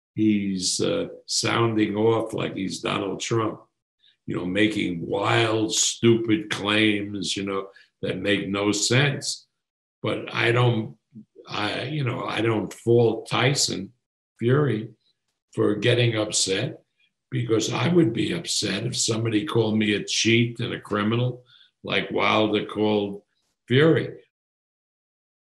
Wilder “sounding off like he’s Donald Trump” says Fury's promoter Arum
Veteran boxing promoter Bob Arum has slammed former WBC heavyweight champion, Deontay Wilder, telling SNTV that "he's sounding off like he's Donald Trump."

After Tyson Fury won Wilder's WBC belt with a 7th round stoppage in February, the 35-year-old American has made baseless claims about the Brit's gloves being loaded and how former trainer Mark Breland may have spiked his water during the bout.

On Wednesday "The Bronze Bomber" sent a Twitter message to Tyson Fury asking the Brit to "honour your agreement for a third fight" but earlier this week "The Gypsy King" said Wilder will "never get a shot" and is determined to move on towards a potential unification clash with UK rival Anthony Joshua in 2021.

Fury's US promoter, Arum, thinks there is as much chance of the third fight happening "as Trump has to win the election."

The 88-year-old CEO of Top Rank confirmed that Wilder's team has approached him to mediate terms for a third fight which was meant to happen in July before Covid-19 pushed it back to a proposed December 19th date that US TV networks couldn't accommodate.

But Fury refused to wait until 2021 to fight again and has shifted his focus onto a homecoming fight, due to be against German heavyweight Agit Kabayel in London on December 5th.  If WBO, WBA and IBF titlist Joshua overcomes Bulgarian Kubrat Pulev a week later then the path is clear for both Brits to square off in early 2021.